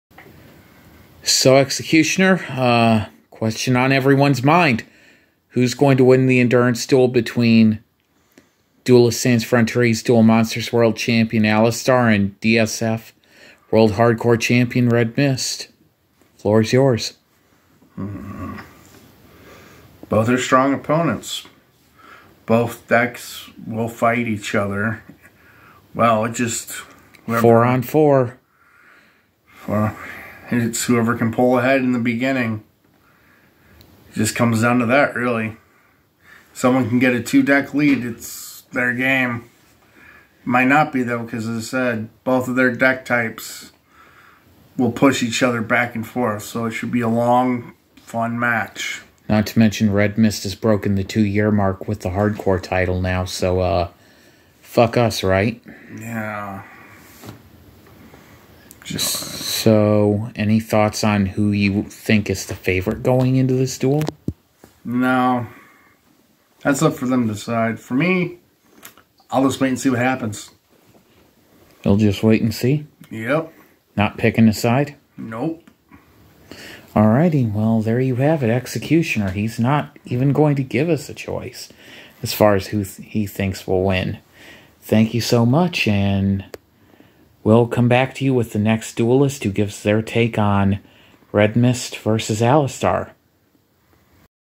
Executioner on Alistar vs RedMyst
Executioner gives his input on the upcoming duel between Duelists Sans Frontières Duel Monsters World Champion Alistar & DSF World Hardcore Champion RedMyst.